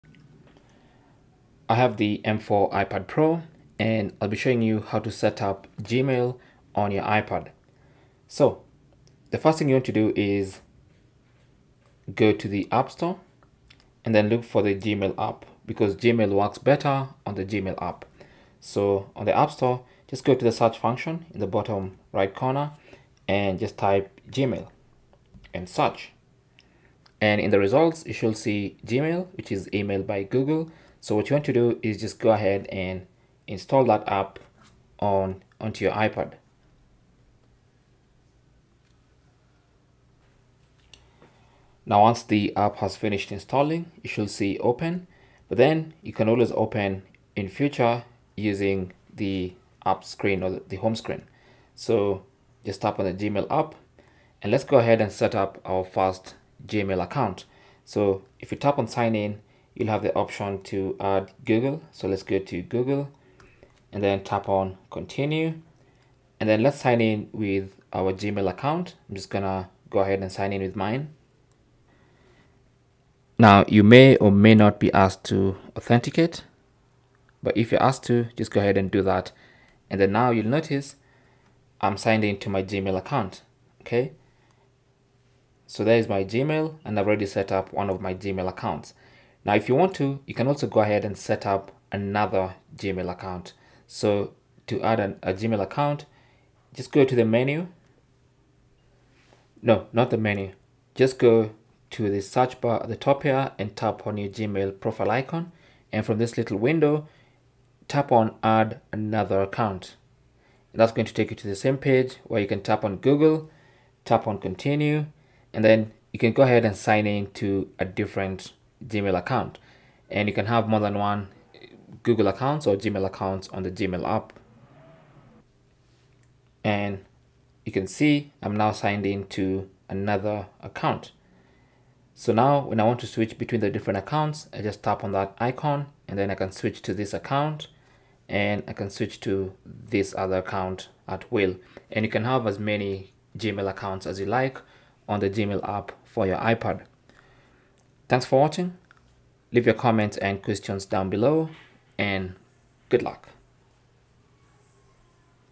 M4 iPad Pro: How To Set Up Gmail | Set Up Multiple Gmail Accounts
Our detailed guide on how to set up Gmail on your M4 iPad Pro. Whether you're new to the iPad ecosystem or just need a refresher, we've got you covered.

Plus, learn how to manage multiple Gmail accounts effortlessly. The M4 iPad Pro is a powerful device perfect for managing emails, multitasking, and staying productive on the go. Make the most out of your device with our easy-to-follow guide.

How To Set Up Gmail On M4 iPad Pro:
How To Set Up Multiple Gmail Accounts On M4 iPad Pro:

Step 1: First, go to the Apple App Store and search for the Gmail app to download and install it if you haven't already.

Step 2: Open the Gmail app from the App Store or from the apps screen. You will be met by a sign in screen. Tap on "Sign In".

Step 3: In the Add Account page, you will see a list of available account types. From the list of account types, select "Google". This will open a new window where you can enter your Gmail credentials after you tap on "Continue" to give permission.

Step 4: Enter your Gmail address and tap "Next". Then, enter your password and tap "Next" again. If you have two-factor authentication enabled, follow the prompts to complete the sign-in process.

Step 5: Tap on "Allow" to be able to receive notifications. Do this as you're being signed in. You will then be brought to your Gmail inbox.

Adding Multiple Gmail Accounts

Managing multiple Gmail accounts on your iPad Pro is just as straightforward. Here’s how you can add additional Gmail accounts:

Step 1: Tap on your Gmail profile icon that is located at the top, on the Search bar. From the resulting window, tap on "Add Another Account".

Step 2: In the Add Account page, you will see a list of available account types. From the list of account types, select "Google". This will open a new window where you can enter a different set of Gmail credentials after you tap on "Continue" to give permission.

Step 3: Enter the credentials for your additional Gmail account and follow the same steps as outlined earlier. Repeat this process for each Gmail account you wish to add.

Step 4: To switch between your Gmail accounts in the Gmail app, tap on the profile icon at the top, on the Search bar. from the window that pops up, you will see a list of all your added accounts. Tap on the account you want to access.

Cell Phone Tripod Adapter